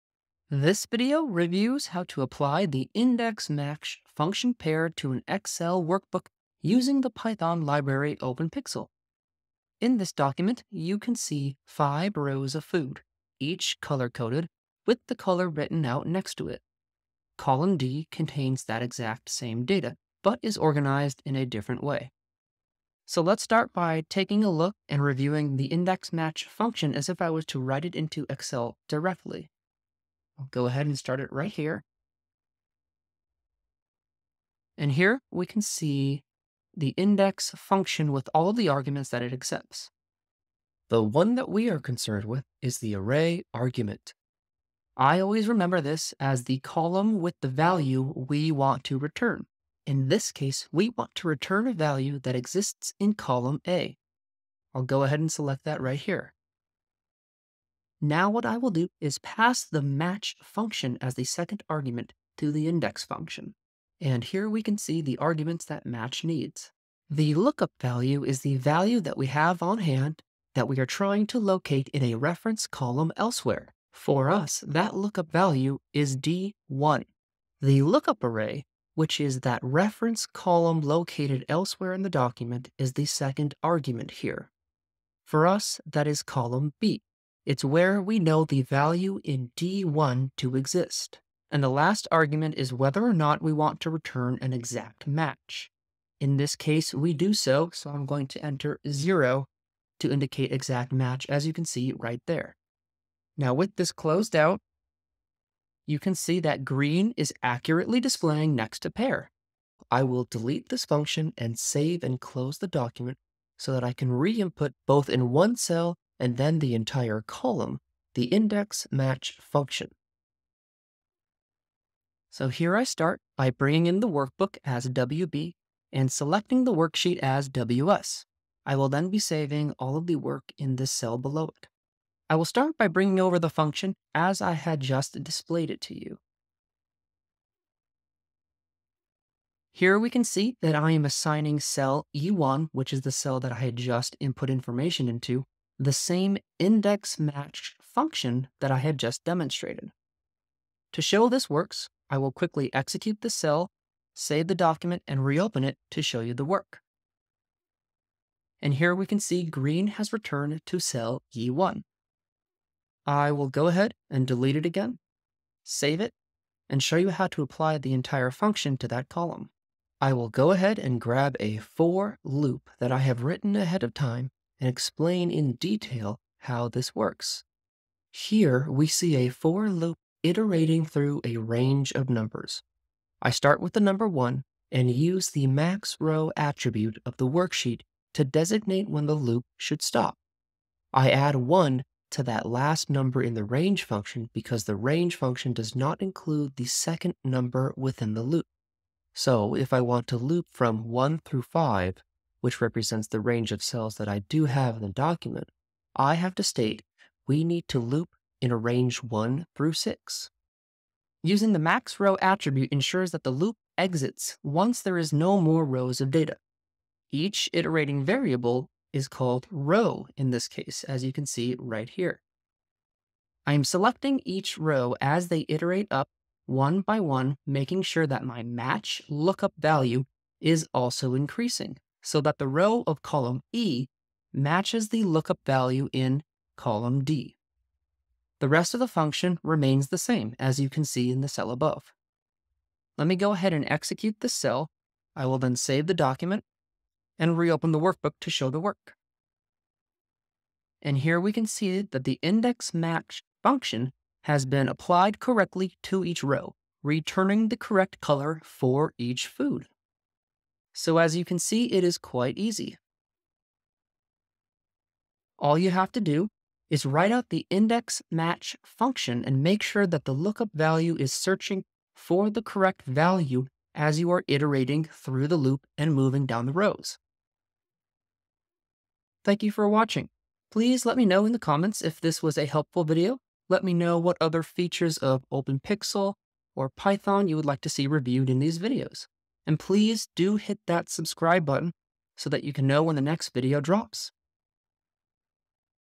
OpenPyXL - INDEX/MATCH Function on an Entire Column in Excel Workbooks with Python | Data Automation
This video demonstrates how to apply the INDEX/MACTH function pair to an excel workbook using the Python library Openpyxl. I use Anaconda's Jupyter Lab interface.

00:00 :: Intro and workbook review
00:22 :: INDEX/MATCH function function/argument review
02:12 :: INDEX/MATCH one cell with Python
02:48 :: INDEX/MATCH entire column with Python
04:20 :: Overview